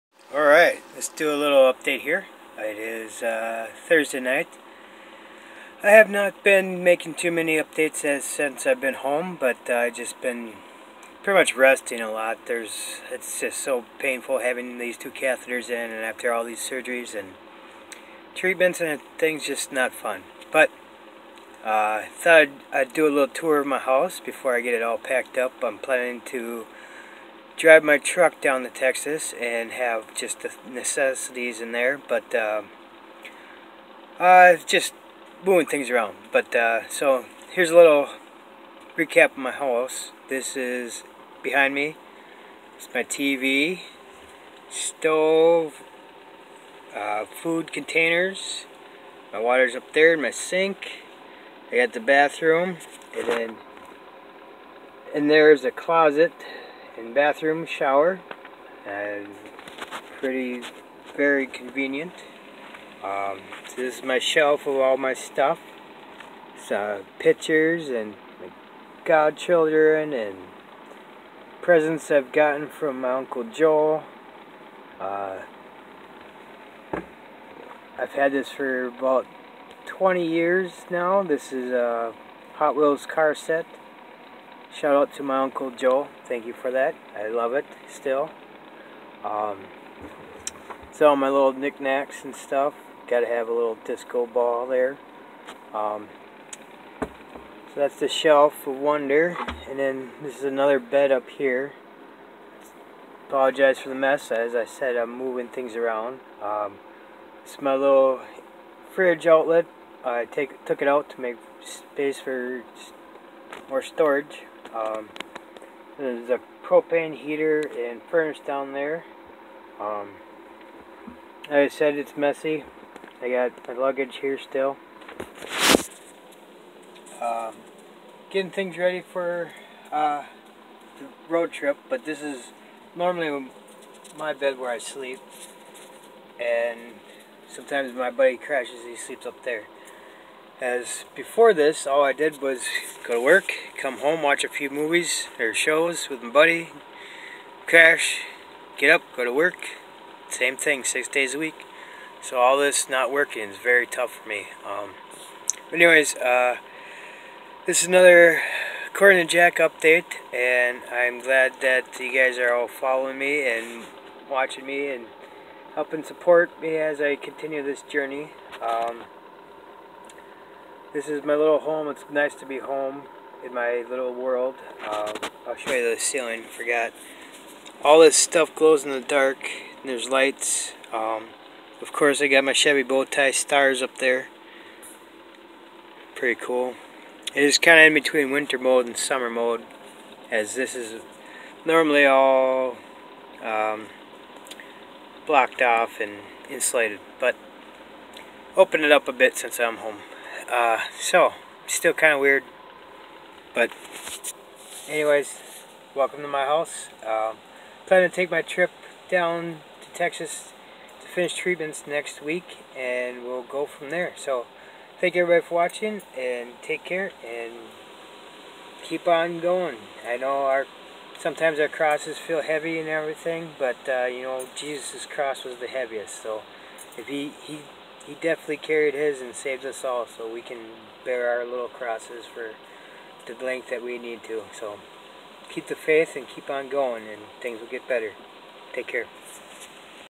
According to JACK home UPdate
It is nice to be home on my break from treatment and surguries in TX. This is a little tour of my tiny home as i talk plans for the future.